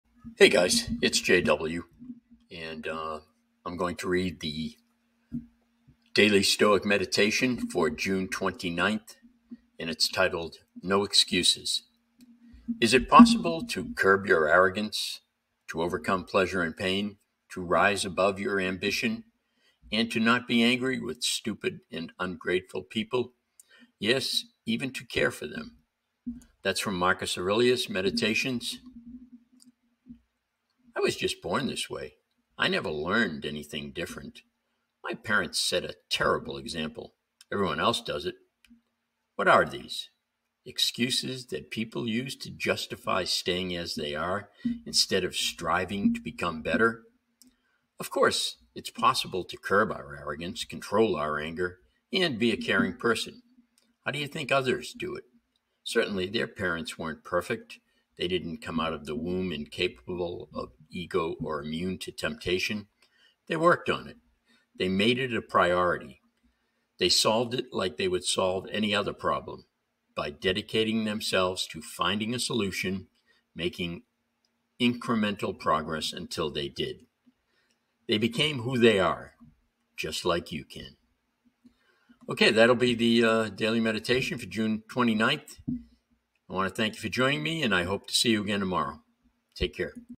Daily Stoic Meditation For June 29th No Excuses
Join us for a transformative Daily Meditation session on June 29th, where we'll teach you how to prioritize self-care and mindfulness without making any excuses. Discover the power of consistency in your meditation practice and unlock a sense of calm and clarity that will carry you through the day. Don't miss out on this opportunity to invest in your well-being - watch the video now!